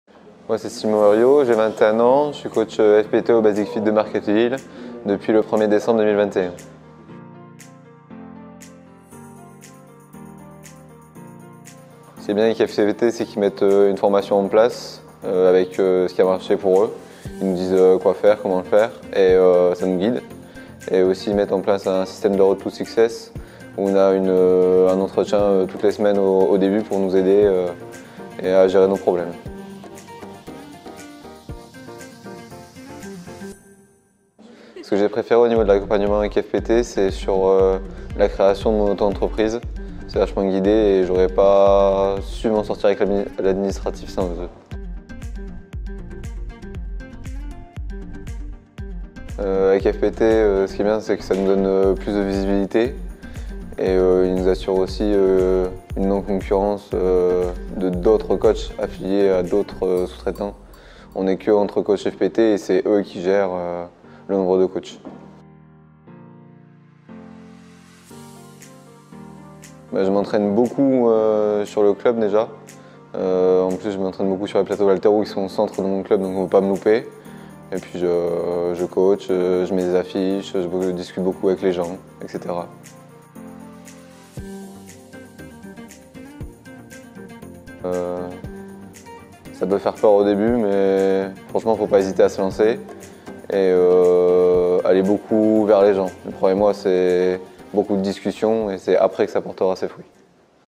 Témoignage Personal Trainer - Simon - Lille
Retrouvez le témoignage de notre Personal Trainer, Simon, qui représente et aide au quotidien ses clients. Une aventure qui lui a permit de grandir et d'être mieux accompagné dans ses démarches de coach indépendant.
Envie de rejoindre le réseau FPT et d'être accompagné à ta juste valeur ?